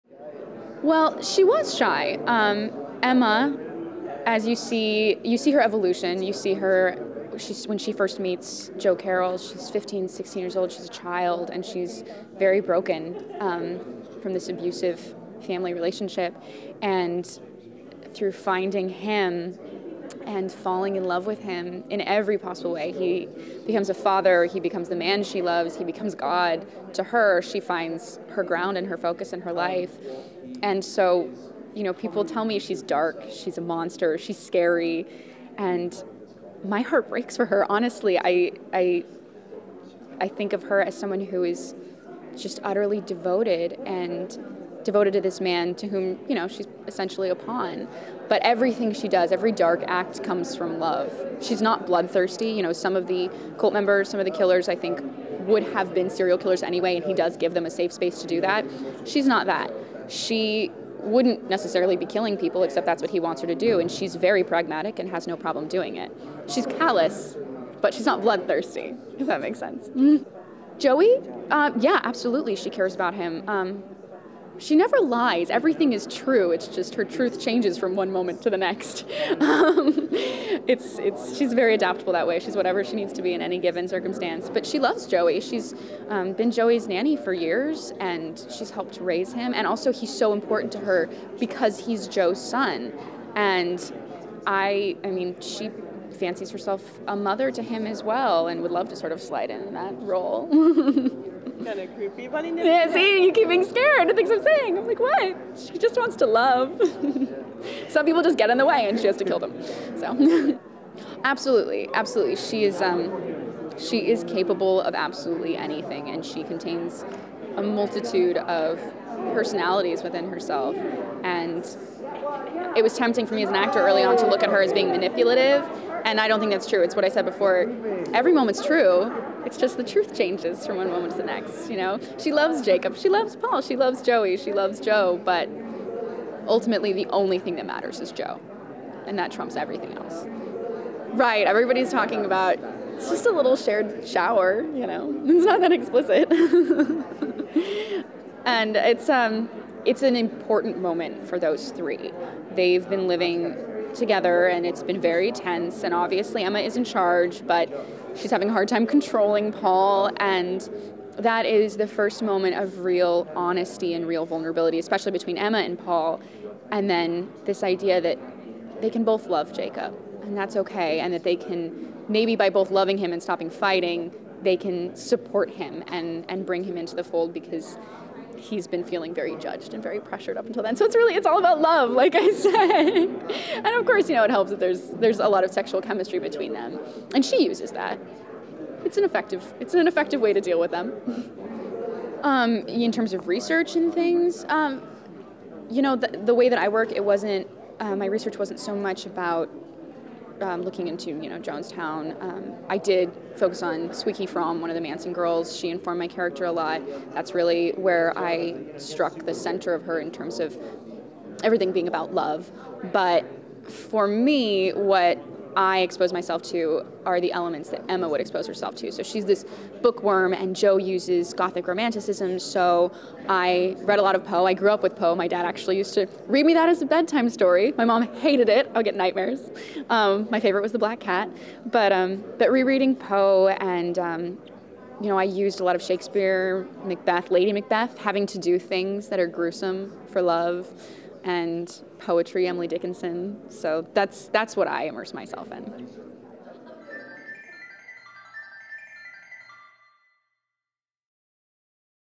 The Following - Valorie Curry - Emma
Valorie Curry talks about Emma's transition from shy girl to killer and what it is that drives her. "She's not blood thirsty... she's very pragmatic." Valorie also talks about Emma's love for Joey and the role she'd like to assume. And she talks about the three way and why it is an important moment for Emma, Paul and Jacob. Lastly, Valorie talks about how she prepared for the role and shares what books her dad used to read to her as bedtime stories! &nbsp;
&nbsp;